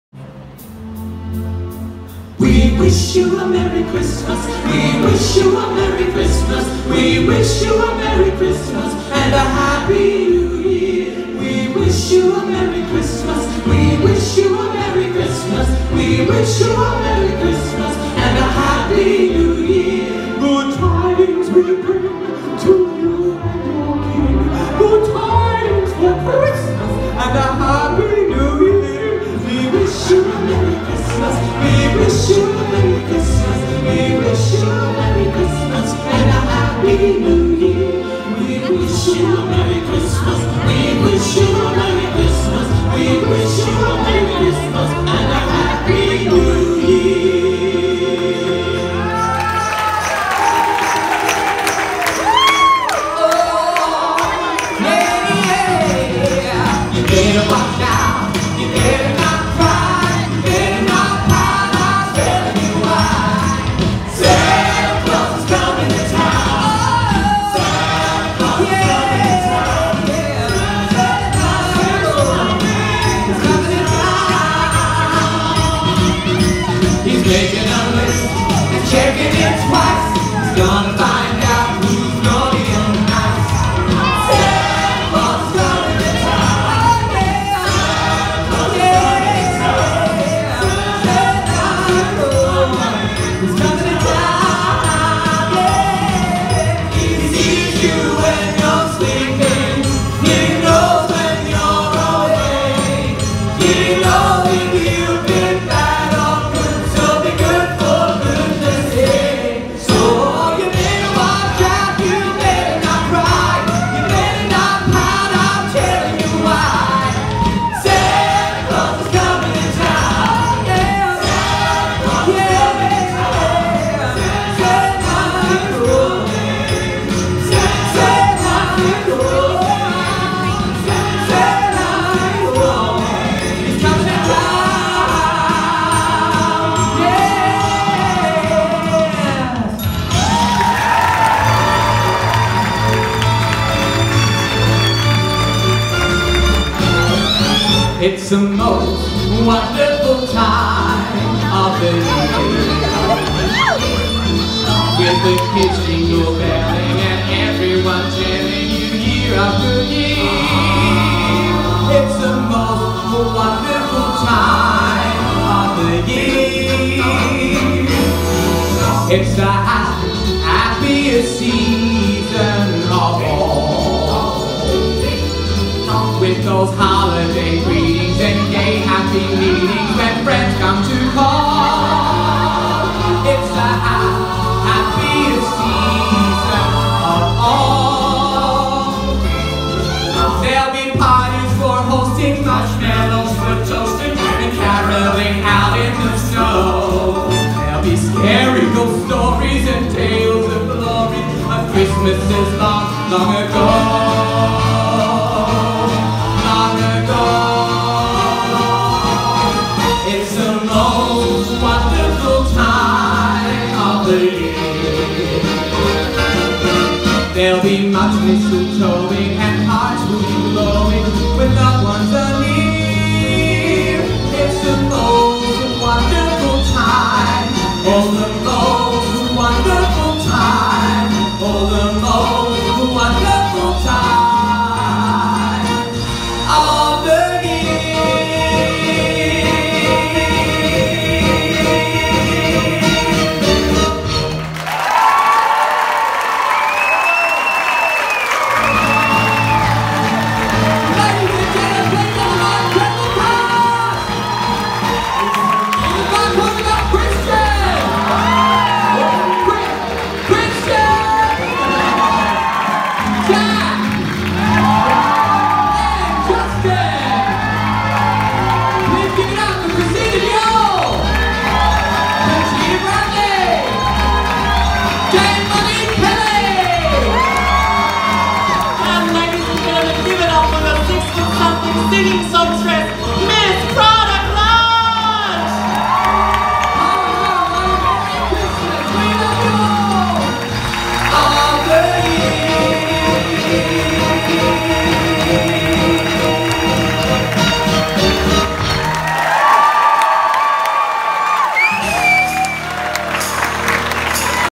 Prada’s Priscillas - Christmas Medley
Prada Clutch and her cast at Prada's Priscilla's sing and perform a medley at the Juniors in Sydney on the 23rd of December 2018 x